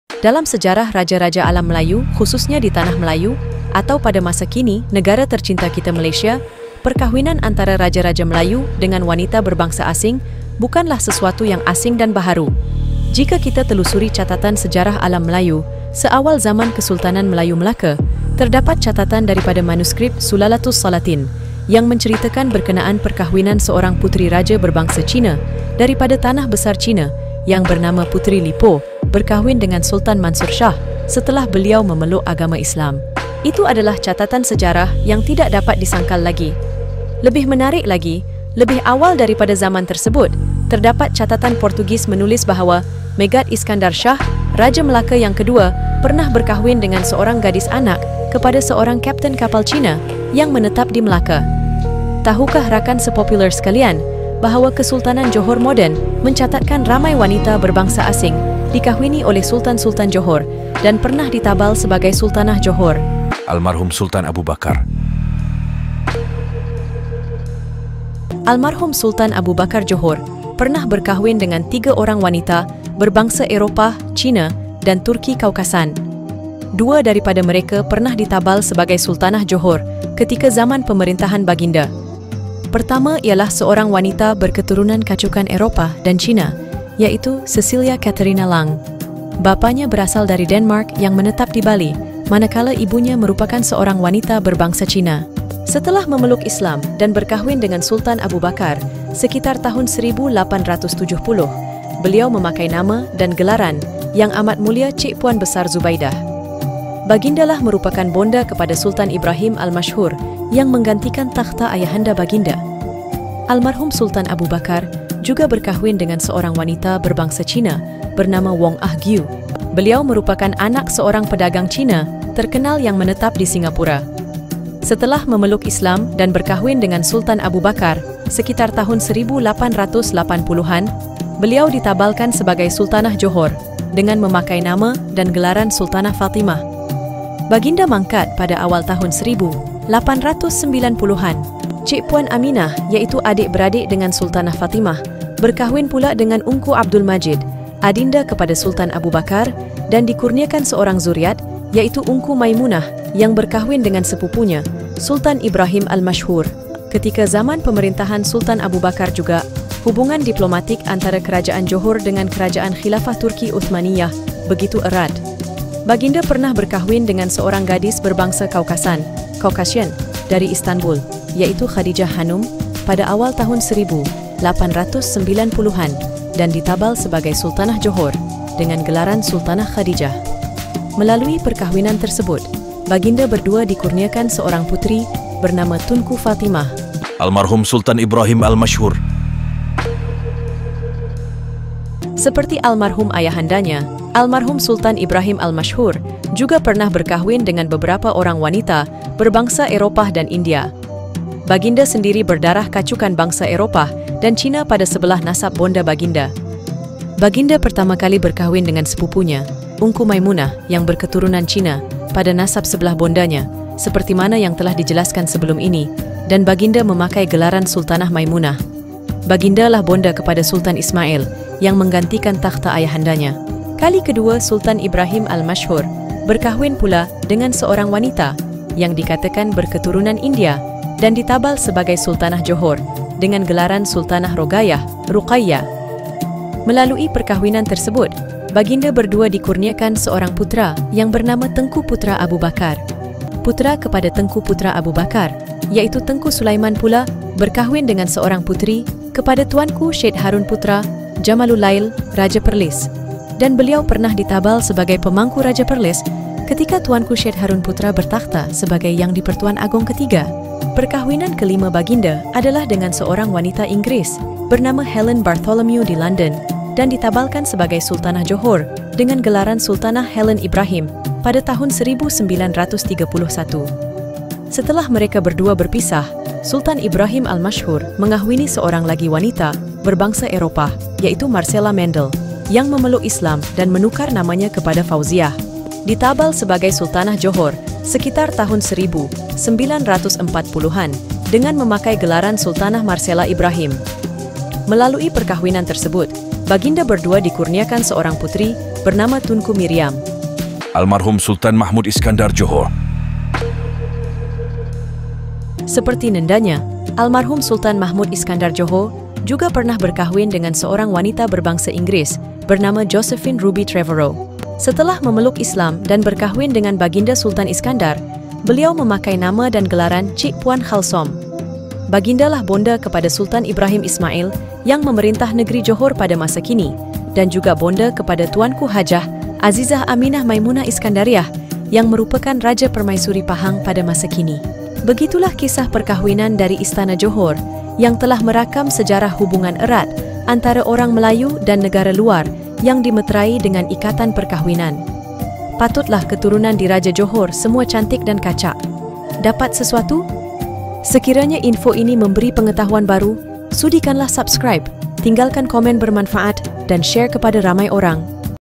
Bukan Sekadar Jadi Isteri Raja! Deretan Wanita BUKAN MELAYU Ini Juga Ditabal Sultanah
Dalam sejarah Raja-Raja Alam Melayu khususnya di Tanah Melayu atau pada masa kini negara tercinta kita Malaysia, perkahwinan antara Raja-Raja Melayu dengan wanita berbangsa asing bukanlah sesuatu yang asing dan baharu. Jika kita telusuri catatan sejarah Alam Melayu seawal zaman Kesultanan Melayu Melaka, terdapat catatan daripada manuskrip Sulalatus Salatin yang menceritakan berkenaan perkahwinan seorang puteri raja berbangsa China daripada Tanah Besar China yang bernama  Puteri Li Po berkahwin dengan Sultan Mansur Shah.

🔥All footage, music & images used belong to their true owners.